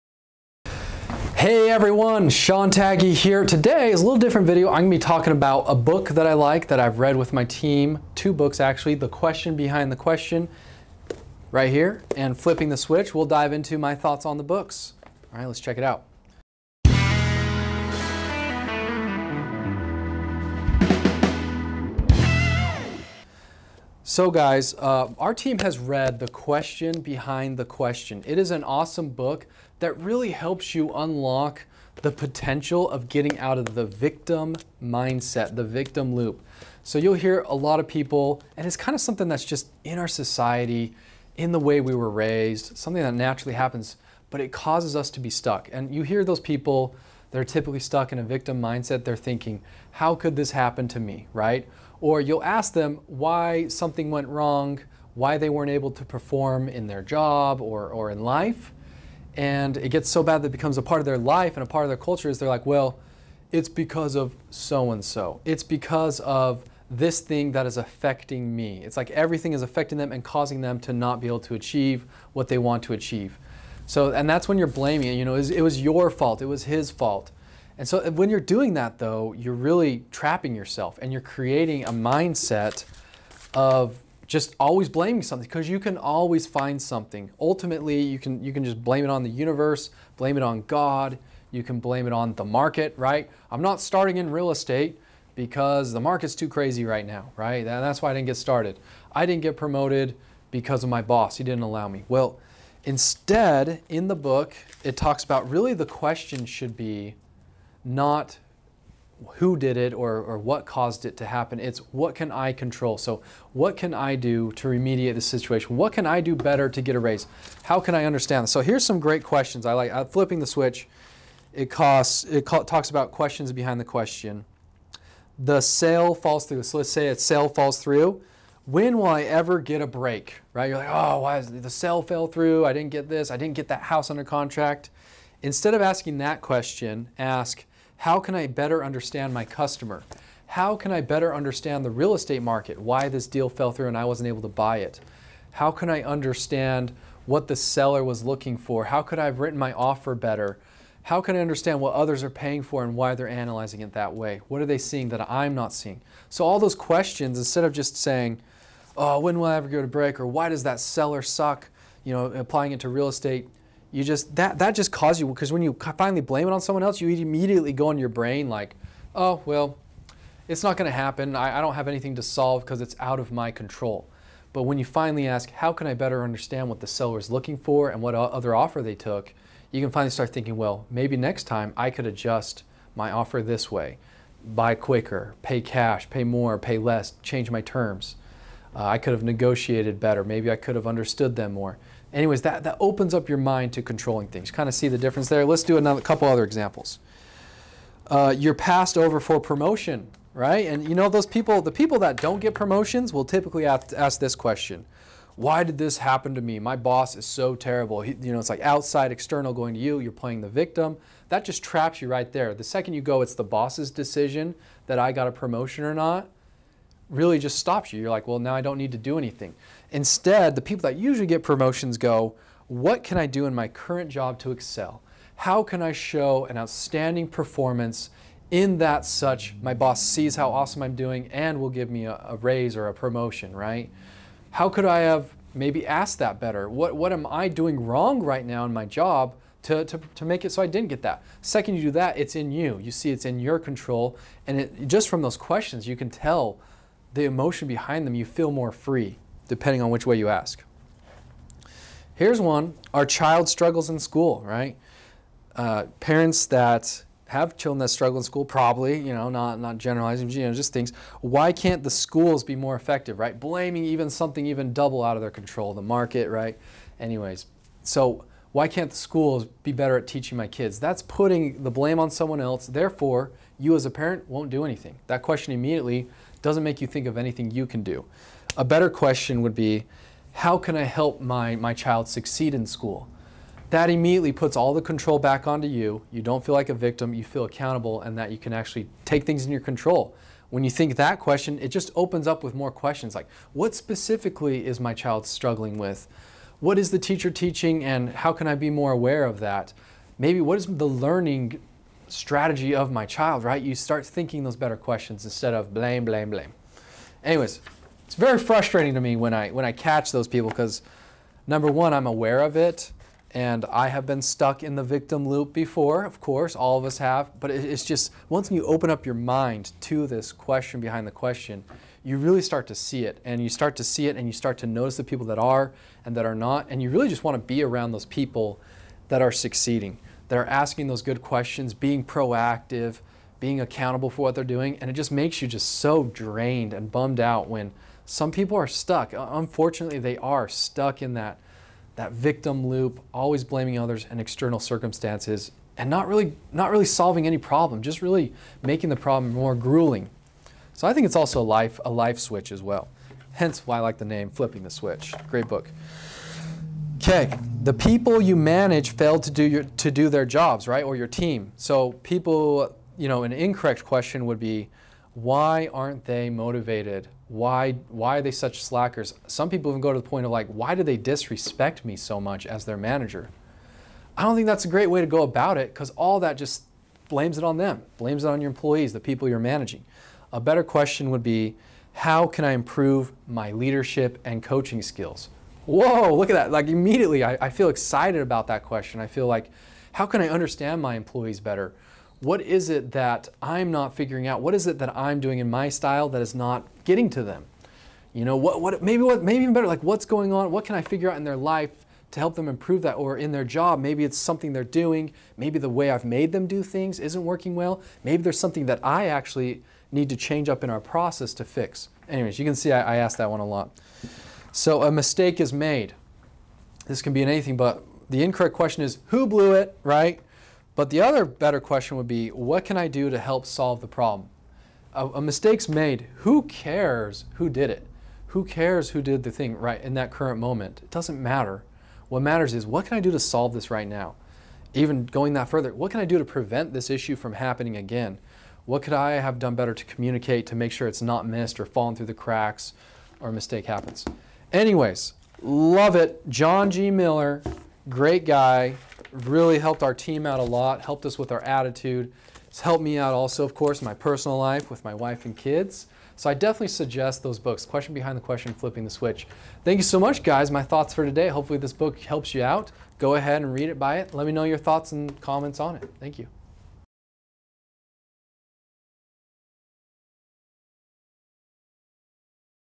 Book Review QBQ
QBQ! The Question Behind the Question
by John G. Miller

Asking the right questions gets you better results.  If the questions you are asking are not moving you forward in your life you need to take a look at what you are asking.

Sean takes a look at two phenomenal books that can really shift your mindset.

➤ Free Turnkey Starter Pack Properties.net/